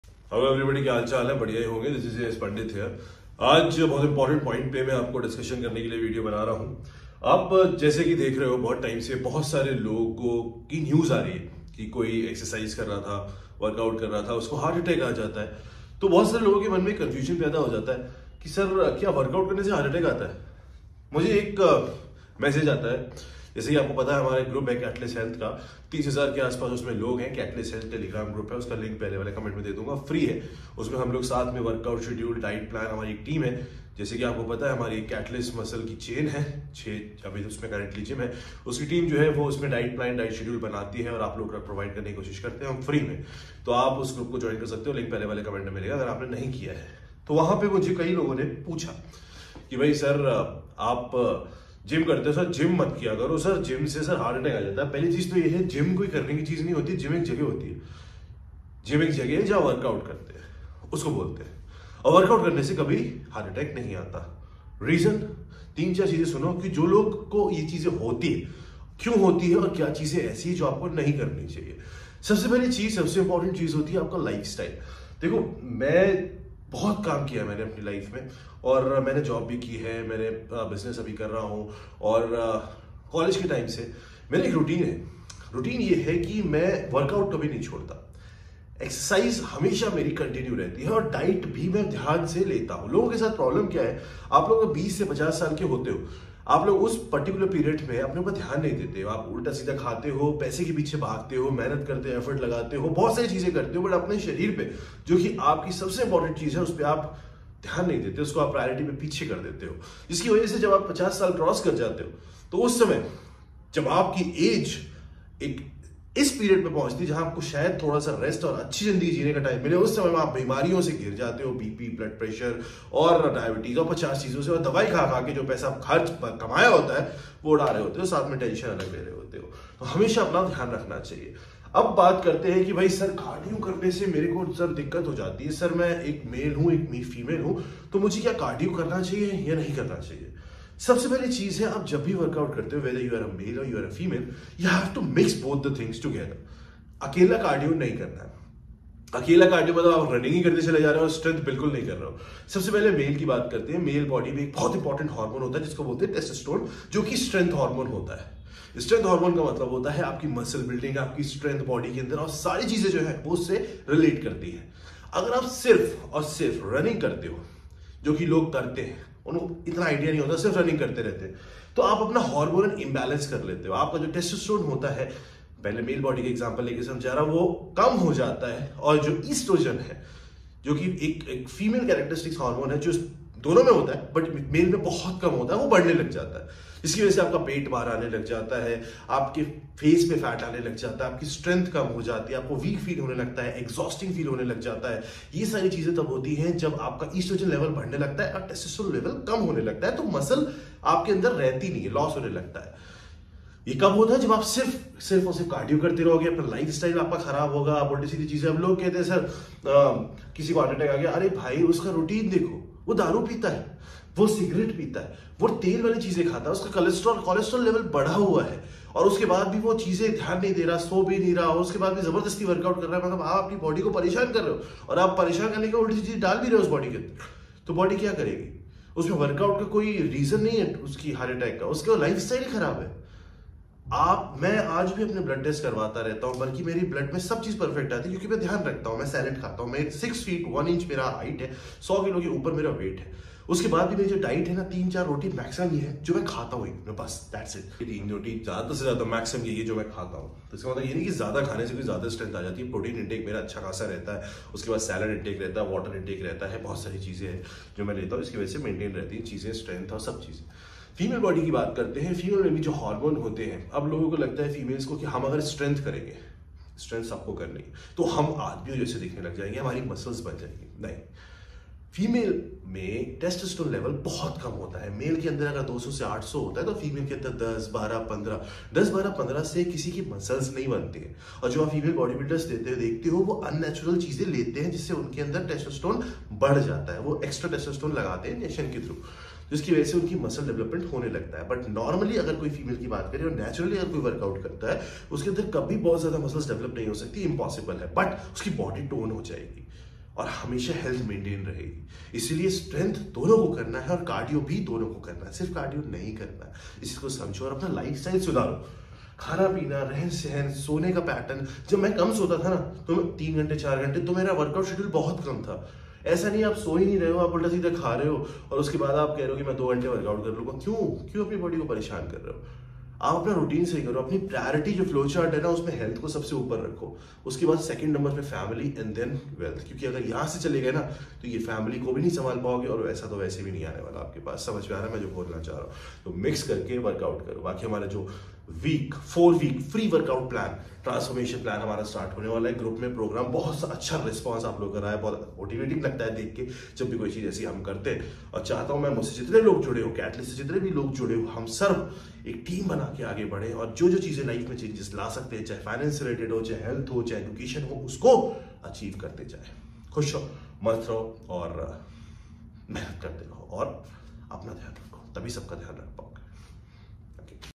Can Exercise & Gym gives you Heart Attack ? Real Details
Gym Exercise , Workout and Heart Attack.Every details discussed . What to do and what Not to Do ?